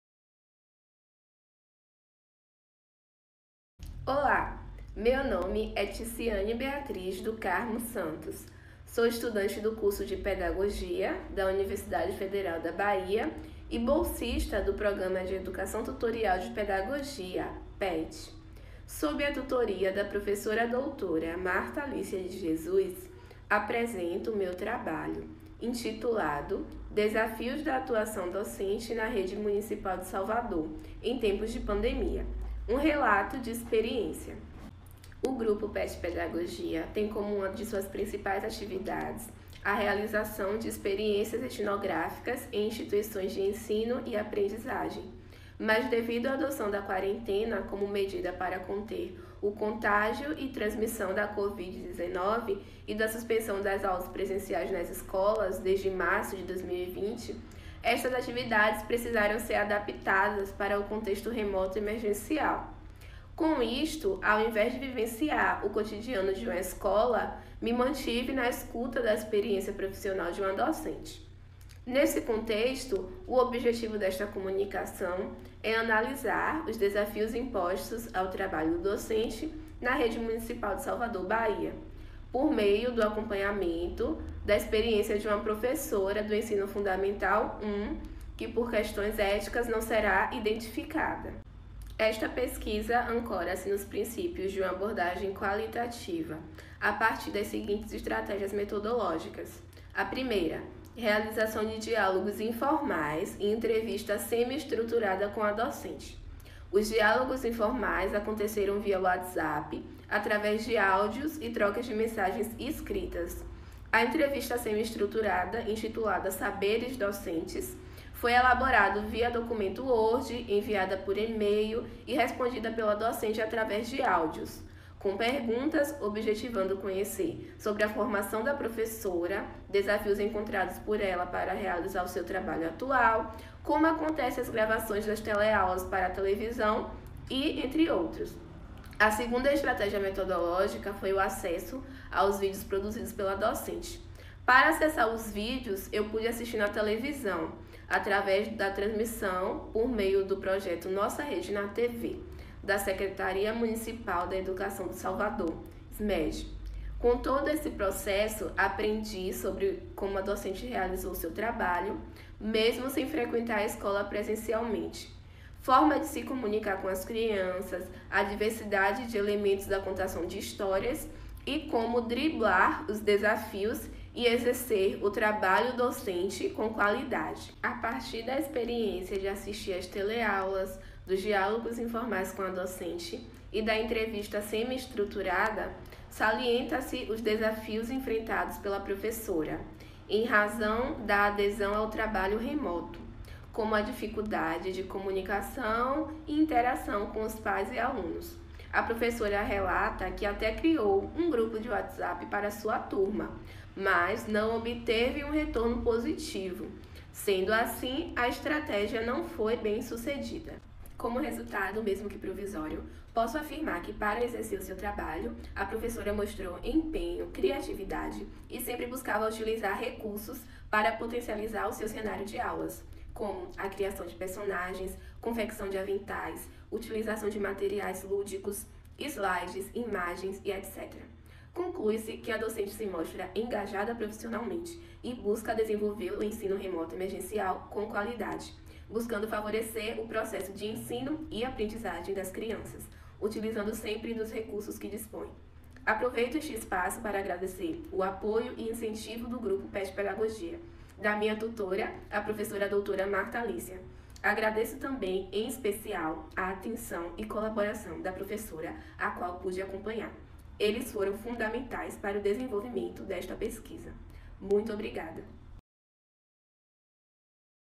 Desafios da atuação docente na rede municipal de Salvador em tempos de pandemia
Título completo: Desafios da atuação docente na rede municipal de Salvador em tempos de pandemia: Um relato de experiência.

Autores:
Ticiane Beatriz do Carmo Santos
Marta Lícia Teles Brito de Jesus

Resumo: Devido à adoção da quarentena como medida para conter o contágio e a transmissão da Covid-19, as escolas tiveram as aulas presenciais suspensas, desde março de 2020. Saraiva, Traversini e Lockmann (2020) salientam que a paralisação das atividades cotidianas escolares ocasionou a transposição das intervenções pedagógicas para o ambiente virtual, através do uso de ferramentas digitais. Diante disso, esta comunicação tem como objetivo analisar os desafios impostos ao trabalho docente na rede municipal de Salvador/BA, no contexto desafiador da pandemia, por meio do acompanhamento da experiência de uma professora do Ensino Fundamental I. A proposta de acompanhar uma docente durante a pandemia surgiu no âmbito das ações do grupo de licenciatura em Pedagogia do Programa de Educação Tutorial (PET). Para delinear as reflexões apresentadas, esta pesquisa ancora-se nos princípios de uma abordagem qualitativa (GIL, 2000), a partir das seguintes estratégias metodológicas: 1) realização de diálogos informais e entrevista semi-estruturada com a docente; 2) acesso aos vídeos produzidos pela docente. A partir da experiência de assistir os vídeos exibidos aos alunos nas teleaulas gravadas do projeto Nossa Rede na TV da Secretária Municipal da Educação do Salvador (SMED), bem como dialogar e entrevistar a professora sobre os desafios enfrentados durante a pandemia para desenvolver sua atividade docente, salienta-se o empenho e a criatividade para desenvolver personagens, confeccionar aventais e, entre outros,  para potencializar o cenário para a contação de histórias dos vídeos por ela produzidos. Destaca-se também a preocupação e os desafios enfrentados para a docente manter a comunicação e a interação com os pais e alunos. Conclui-se que a docente se mostra engajada profissionalmente e disposta a continuar desempenhando seu papel importante  no processo de ensino-aprendizagem das crianças, mostrando-se otimista e com coragem busca estratégias para desenvolver com qualidade o ensino remoto emergencial, com os recursos que dispõe.

Covid-19 Docência EducaçãoBásica SMED PET.